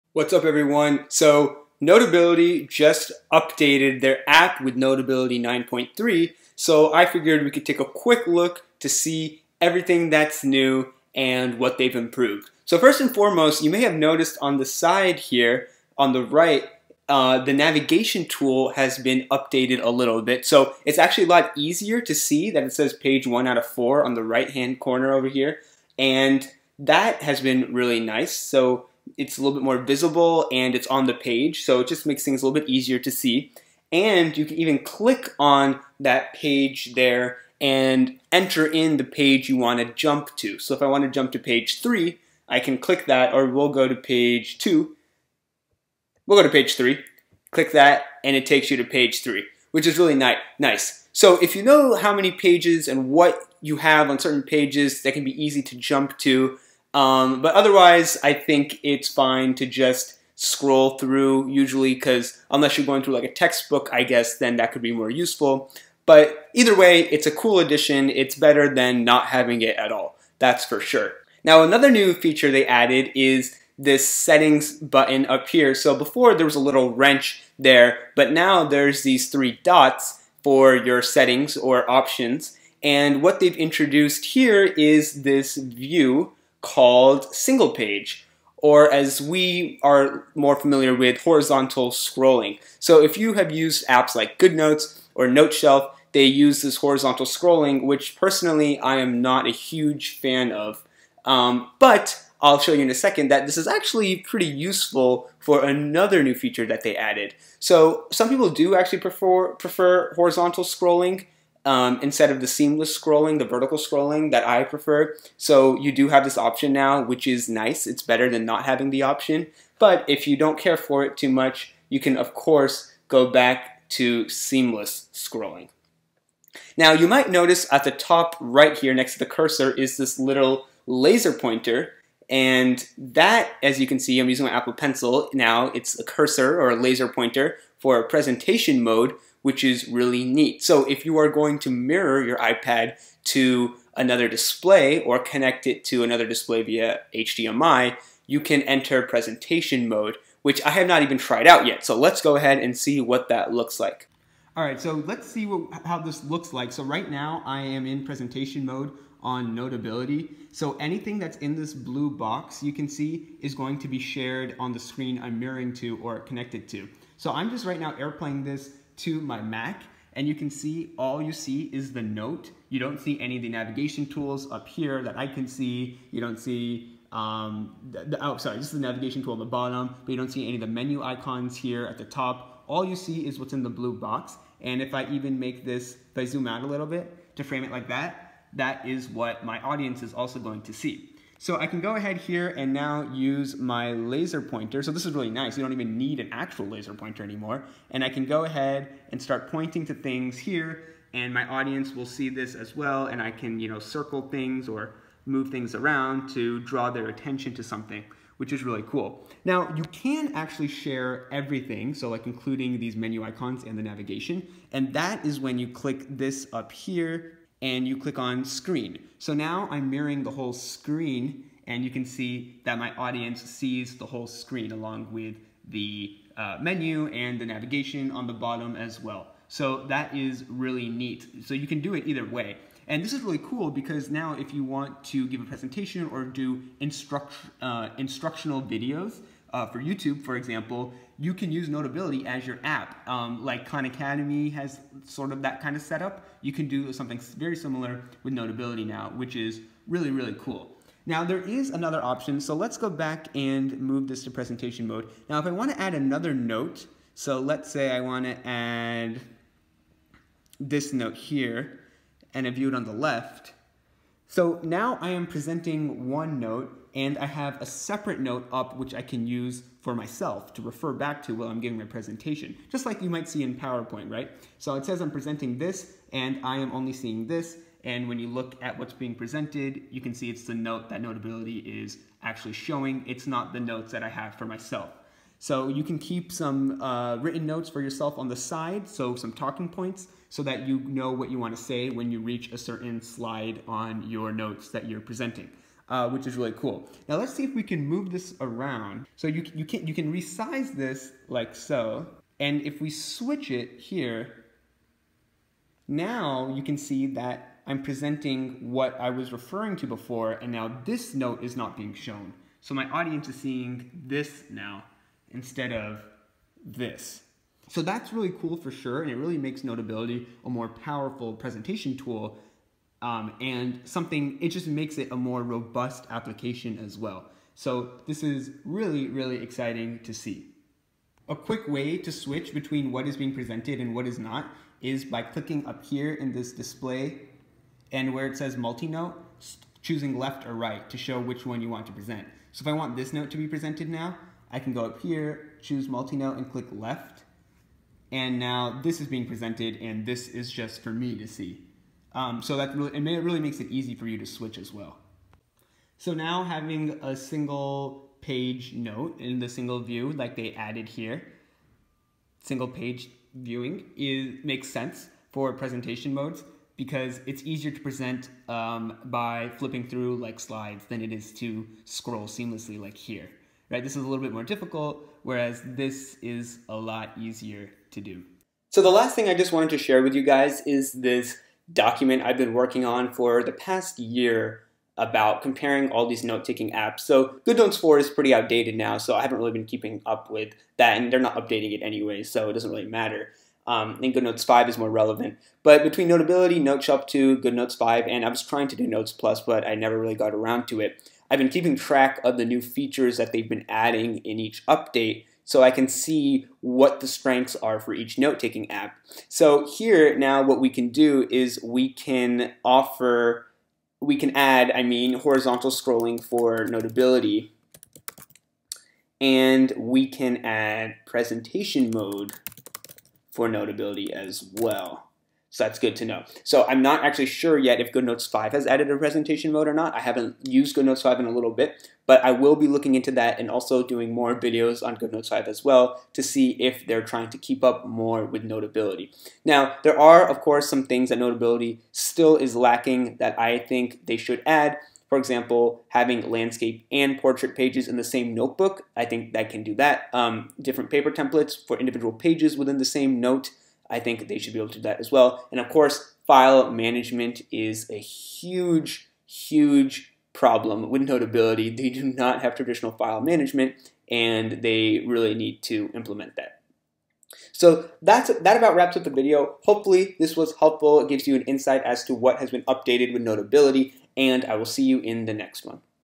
Notability 9.3 March 2020 Update!
New updates to Notability! Great features have been added including presentation mode, horizontal scrolling, and a laser pointer